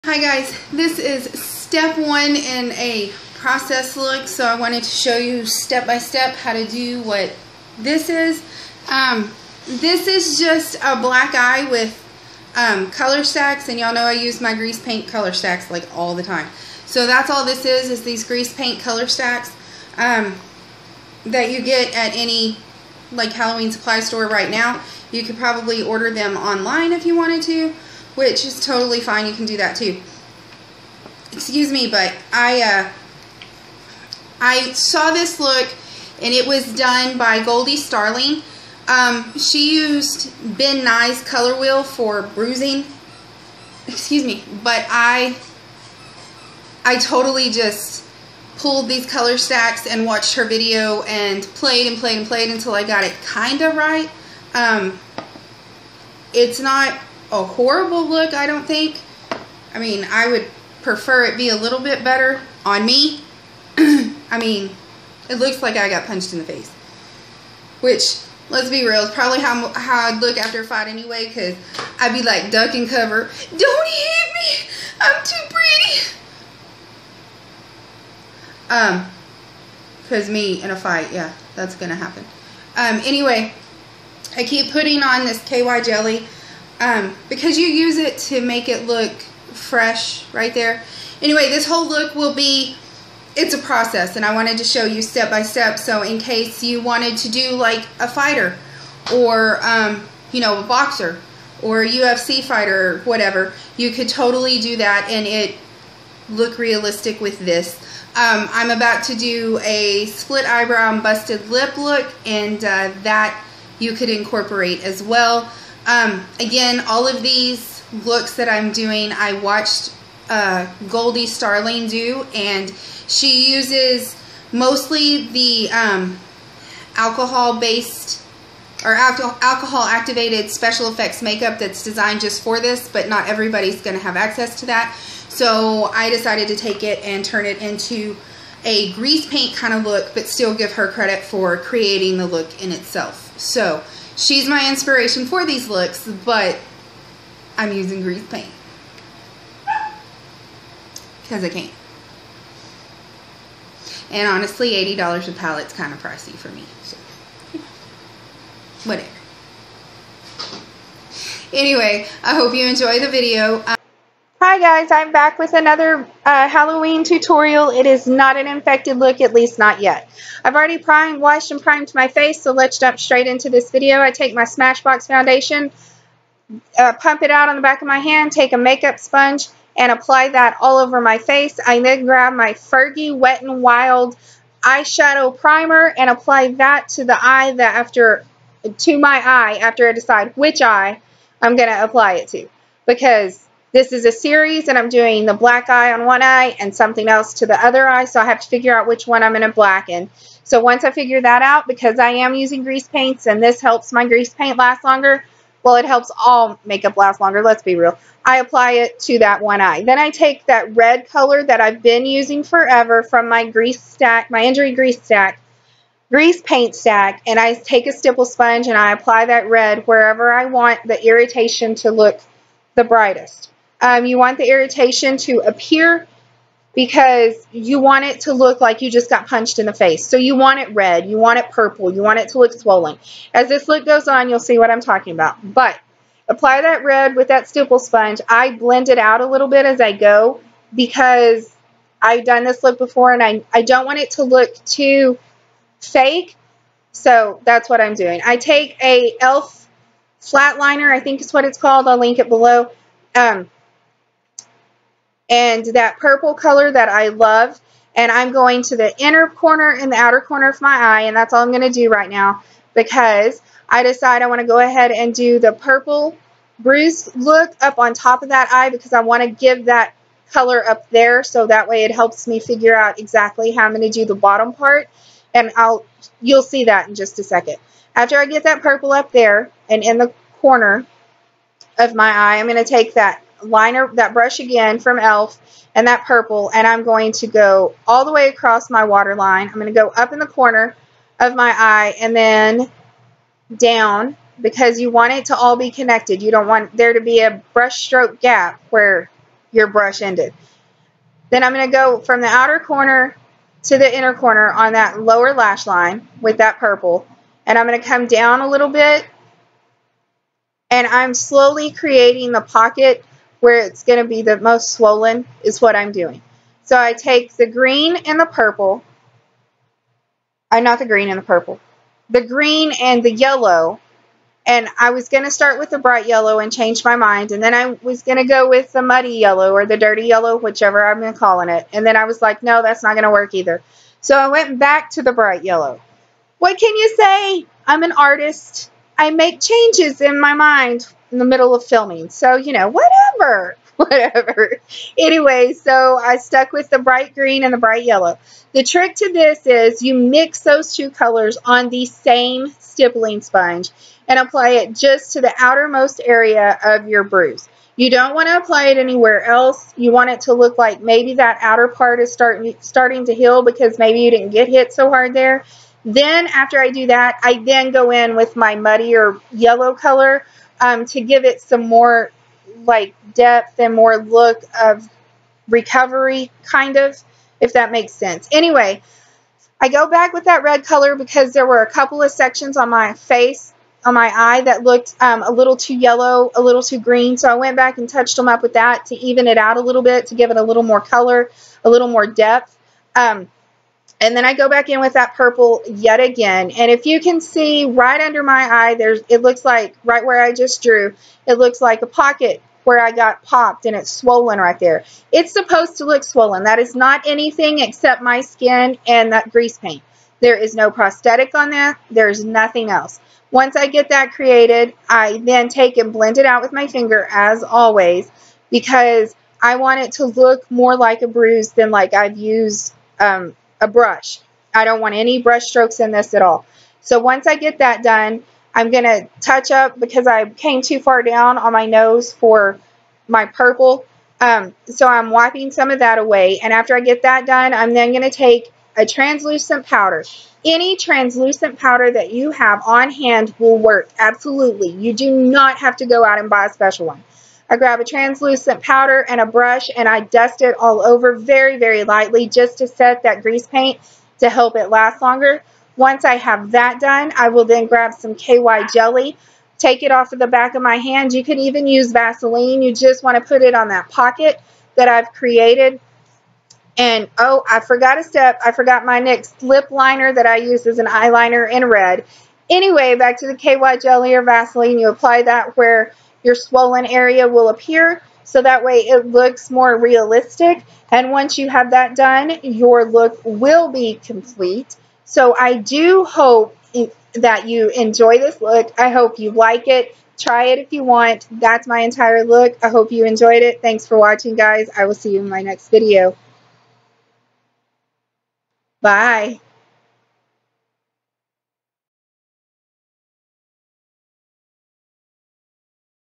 MMA/UFC/Boxer Black Eye Halloween Special Effects Makeup Tutorial
The black eye in this tutorial was inspired by me watching a WONDERFUL FX artist here on youtube, goldiestarling. I chose to recreate this look using grease paint stacks from the local Halloween supply store, because that is easier and faster for most of us to get on short notice.

The products used:
Smashbox Halo Foundation in 1
Wet n WIld Fergie Eyeshadow Primer
Makeup Forever Translucent setting powder
Grease paint injury stack colors in: Deep Red, Burgundy (deep purple), Dirty yellow (that's what I call it) and any green and yellow